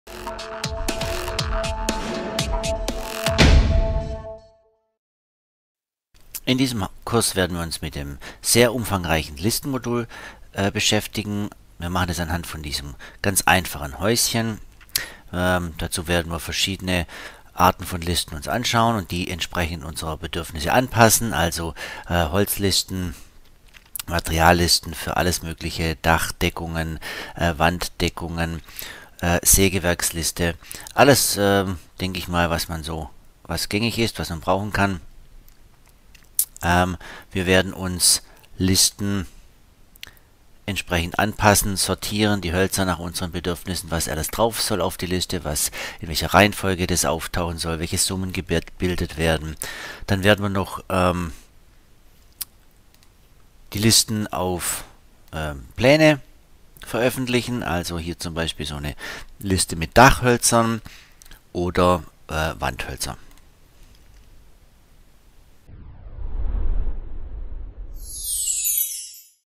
Online Kurs: Listen
Das umfangreiche Thema Liste / Materiallisten wird am Beispiel eines Einfamilienhaus durchgenommen. Im Videokurs werden verschiedene Arten von Listen erklärt und wie sich diese nach den eigenen Bedürfnissen anpassen lassen wie etwa Holzliste, Materiallisten für Dach oder Wand mit den jeweiligen Deckungen. Die Sägewerksliste und alle weiteren gängigen Listen werden ebenfalls besprochen. Wie man Listen auf Pläne anwendet, erfahrt ihr zum Ende des Kurses.

Der Online-Kurs wird knapp 1,5 Std. Zeit in Anspruch nehmen. Alle Einzelvideos werden im SEMA College, der Lernplattform zum eigenständigen Erlernen der SEMA Software bereitgestellt in der Rubrik "SEMA Lernen von A bis Z"

• Erfahren Sie mehr über SEMA Software und unsere Innovationen